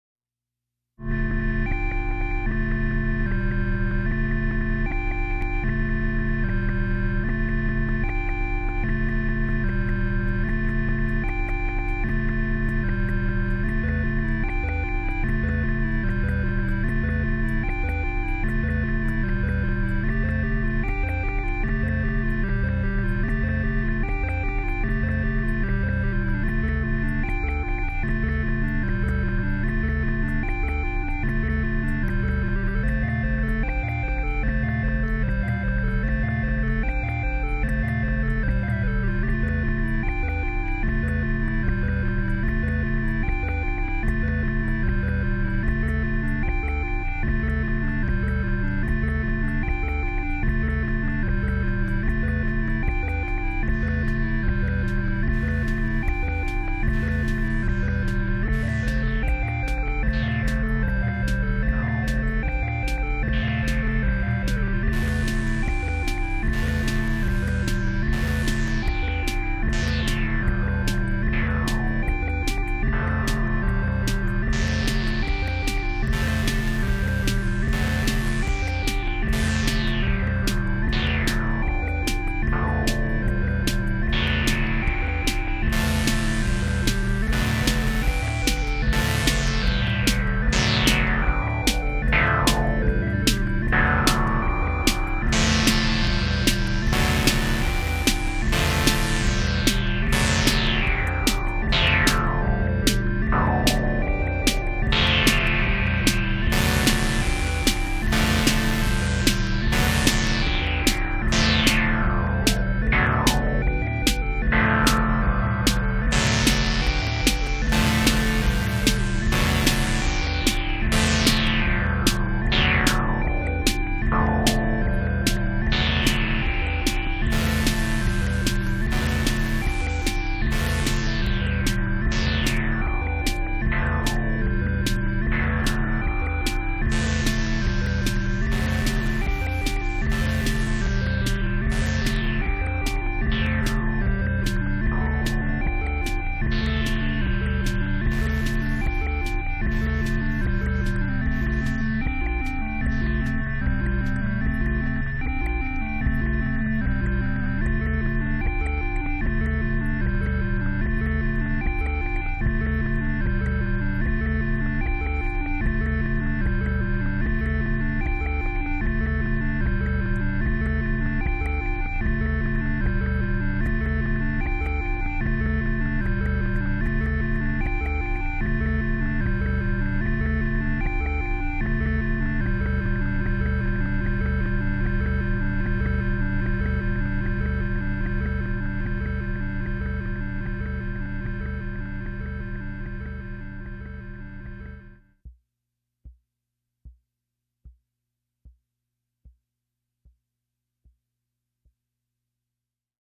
PunkClock 030722 ( Moog Subharmonicon, Roland SH-01A, Behringer RD-6 )
Observe the Moog Subharmonicon playing a Sequence. It is then joined by Hats from the Behringer Rd-6 & offers us a 2nd Sequence as the RD-6 introduces more Percussive Voices. Finally some squelchy Chords from the Roland SH-01A... a bit of manipulation & the toast has popped up. I like mine with Unsalted Butter & some Lemon Curd. How about you?